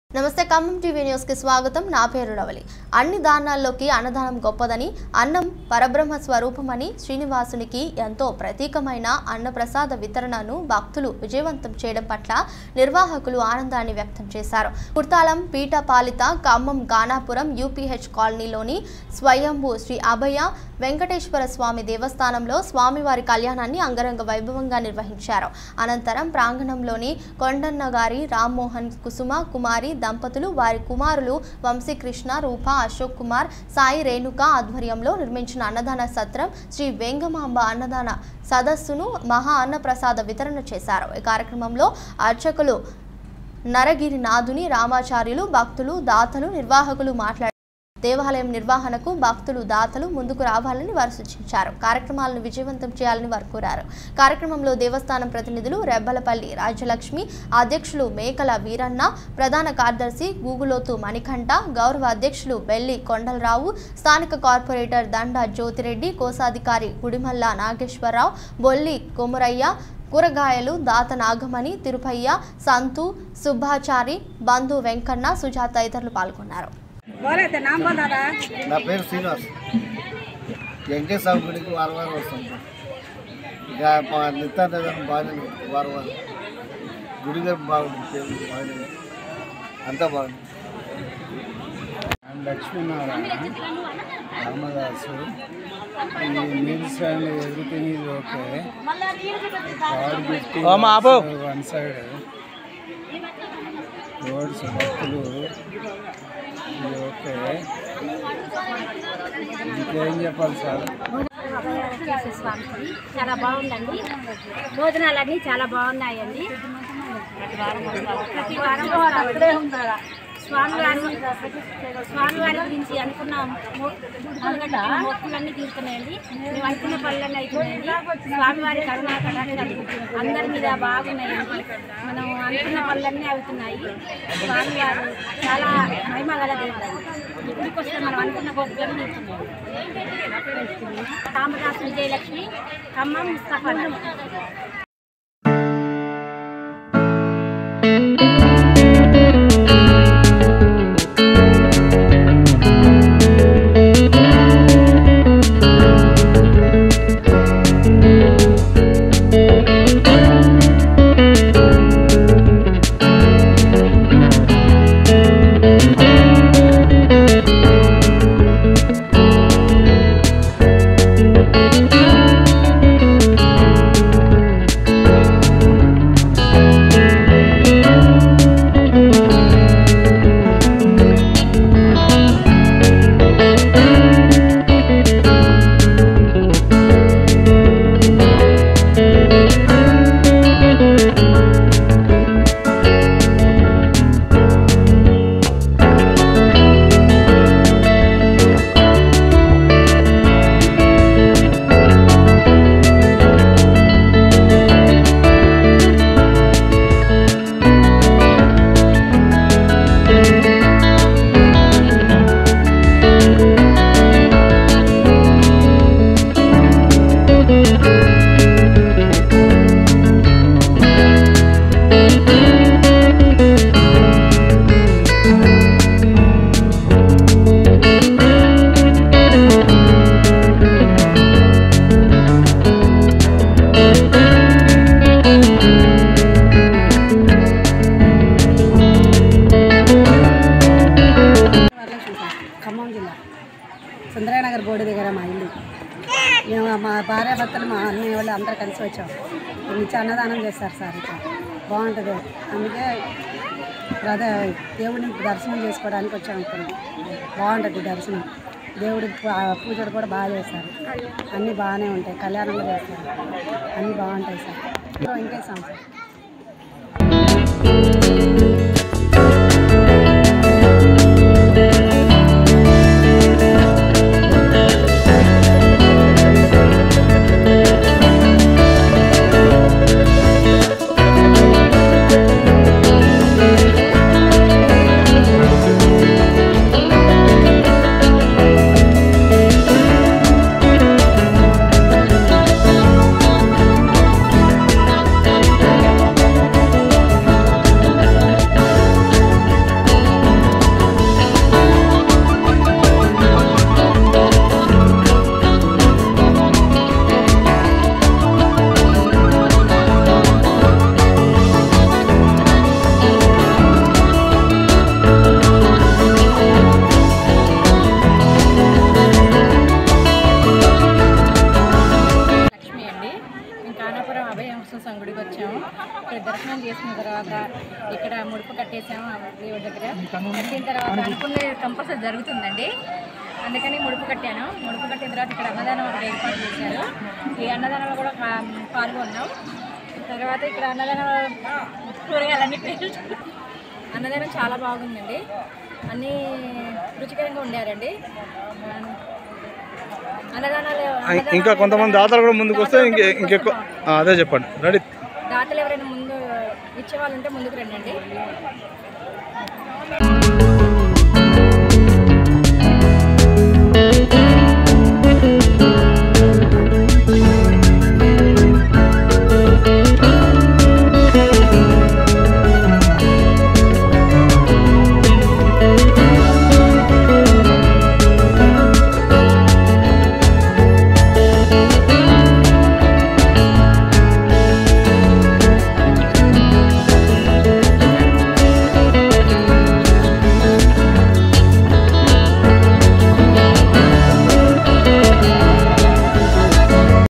అభయ వేంకటేశుని సన్నిదిలో అన్న ప్రసాద వితరణ Distribution of prasada in the presence of Abhaya Venk
అన్ని దానాల్లోకి అన్నదానం గొప్పదనీ, అన్నం పరబ్రహ్మ స్వరూపమనీ, శ్రీనివాసునికి ఎంతో ప్రీతికరమైన అన్న ప్రసాద వితరణను భక్తులు
విజయవంతం చేయడం పట్ల నిర్వాహాకులు ఆనందాన్ని వ్యక్తం చేశారు.
కుర్తాళం పీఠ పాలితా, ఖమ్మం ఖానాపురం UPH కాలనీలోని స్వయంభూ శ్రీ అభయ వెంకటేశ్వర స్వామి దేవస్థానంలో స్వామి వారి కల్యాణాన్ని అంగరంగ వైభవంగా నిర్వహించారు.
అనంతరం ప్రాంగణంలోని కొండన్నగారి రామమోహన్ కుసుమ కుమారి దంపతులూ, వారి కుమారులు వంశీకృష్ణ రూపా, అశోక్ కుమార్ సాయి రేణుక ఆధ్వర్యంలో నిర్మించిన అన్నదాన సత్రం శ్రీ వేంగమాంబ అన్నదాన సదనంలో మహా అన్న ప్రసాద వితరణ చేశారు.
ఈ సందర్బంగా అర్చకులు నరగిరినాధుని రామాచార్యులు, భక్తులూ, దాతలూ, నిర్వాహాకులు మాట్లాడారు.
దేవాలయం నిర్వహణ కు భక్తులూ, దాతలు ముందుకు రావాలని వారు సూచించారు.
కార్యక్రమాలను విజయవంతం చేయాలని వారు కోరారు.
కార్యక్రమంలో దేవస్థానం ప్రతినిధులు రెబ్బలపల్లి రాజ్యలక్ష్మీ, అధ్యక్షులు మేకల వీరన్నా, ప్రధాన కార్యదర్శి గుగులోత్ మణికంఠా, గౌరవ అధ్యక్షులు బెల్లి కొండల్ రావు, స్థానిక కార్పొరేటర్ దండా జ్యోతి రెడ్డీ, కోశాధికారి గుడిమళ్ల నాగేశ్వరావు, బొల్లి కొమరయ్యా, కూరగాయల దాత నాగమణీ, తిరపయ్యా, సంతూ, సుబ్బాచారీ, బంధు వెంకన్నా, సుజాత తదితరులు పాల్గొన్నారు